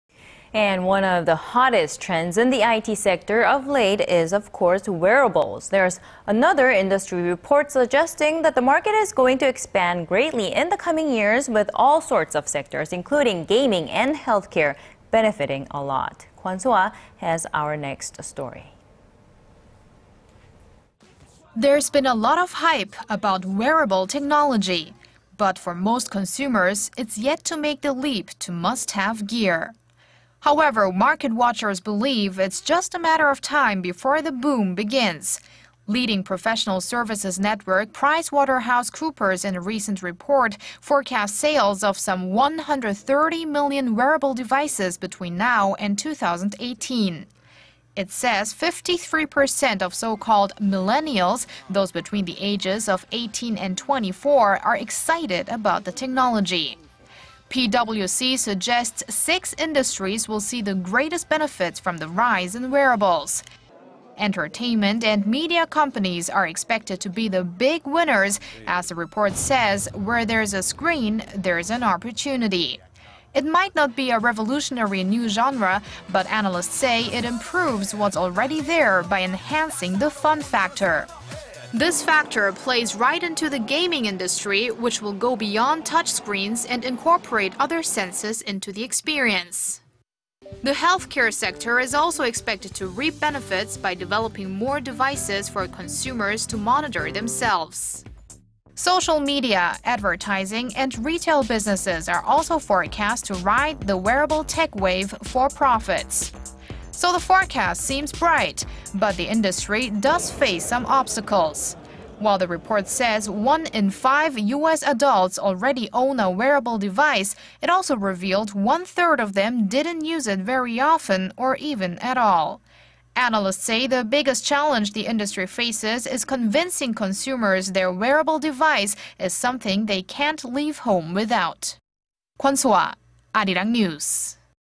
Wearable technology to especially sell well in six industries ′웨어러블′시대 유망업종은?
And one of the hottest trends in the IT sector of late is wearables.
There′s another industry report suggesting that the market is going to expand greatly in the coming years... with all sorts of sectors including gaming and health care... benefitting a lot.
Kwon Soa has our next story.
There′s been a lot of hype about "wearable technology" but, for most consumers, its yet to make the leap to must-have gear.
However, market watchers believe it′s just a matter of time before the boom begins.
Leading professional services network, PricewaterhouseCoopers, in a recent report, forecast sales of some 1-hundred-30 million wearable devices between now and 2018.
It says 53 percent of so-called ′millennials′, those between the ages of 18 and 24, are excited about the technology.
PwC suggests six industries will see the greatest benefits from the rise in wearables.
Entertainment and media companies are expected to be the big winners... as the report says "where there′s a screen, there′s an opportunity."
It might not be a revolutionary new genre, but analysts say it improves what′s already there... by enhancing the fun factor.
This factor plays right into the gaming industry, which will go beyond touch screens and incorporate other senses into the experience.
The healthcare sector is also expected to reap benefits by developing more devices for consumers to monitor themselves.
Social media, advertising, and retail businesses are also forecast to ride the wearable tech wave for profits.
So the forecast seems bright, but the industry does face some obstacles.
While the report says one in five U.S. adults already own a wearable device, it also revealed one-third of them didn′t use it very often... or even at all.
Analysts say the biggest challenge the industry faces is convincing consumers their wearable device is something they can′t leave home without.